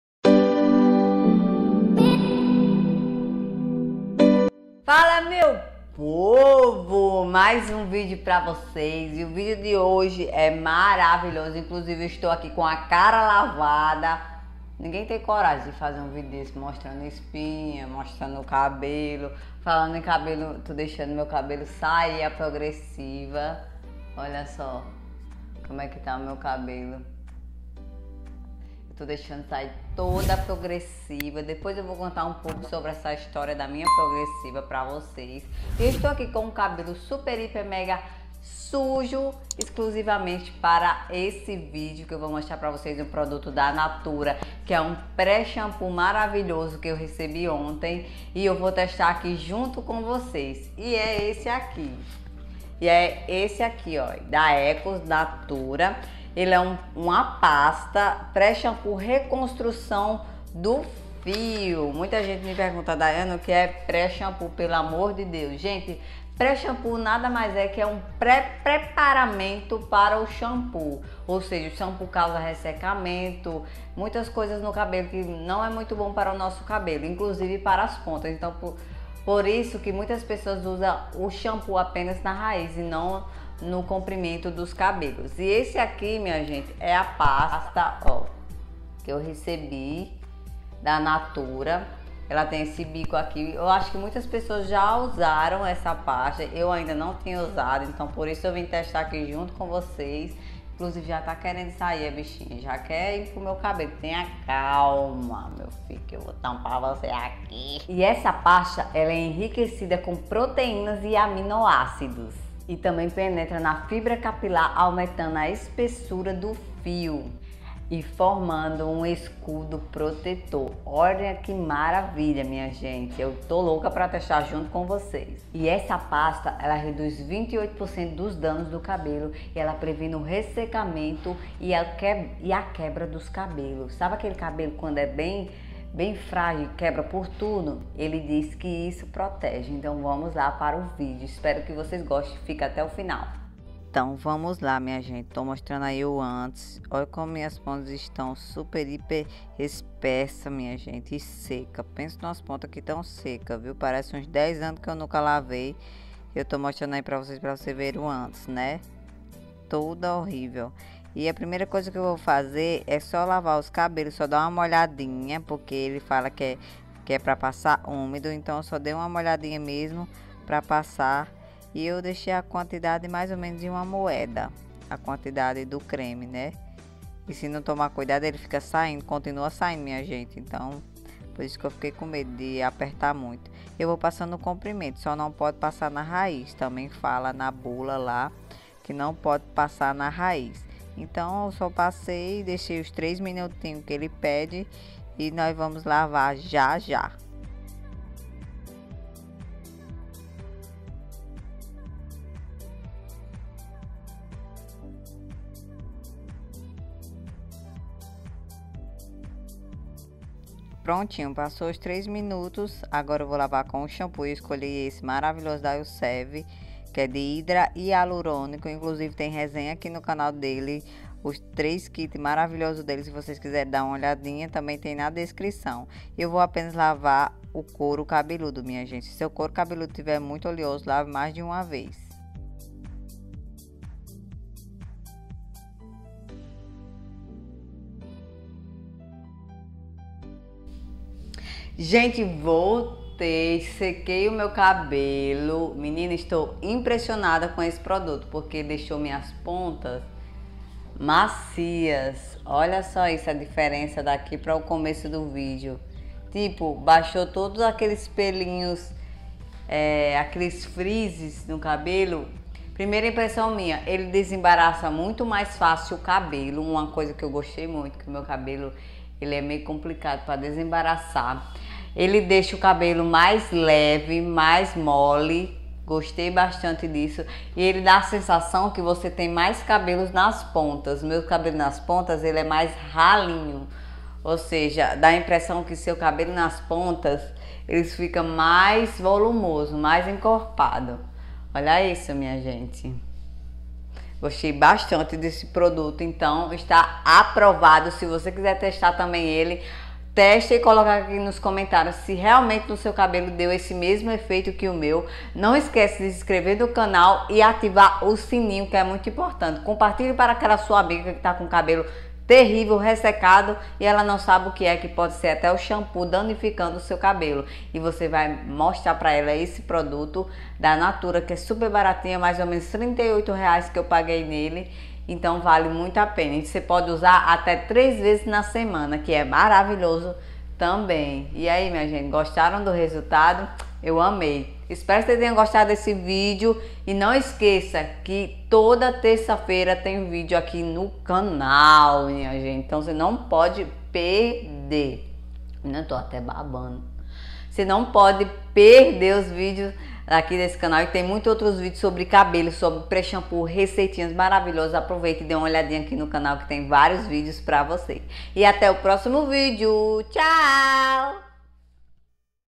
PRÉ-SHAMPOO NATURA EM PASTA MARAVILHOSO(amei o resultado)
E falando especificamente da pasta pré-shampoo, ela é enriquecida com proteínas e promete penetrar no interior da fibra capilar, aumentando a espessura do fio e formando um escudo protetor. Promete também ajudar no desembaraço dos fios úmidos ou secos.

VEJA MAIS VÍDEOS AQUI🔽

GOSTA DE PRODUTOS DE BELEZA?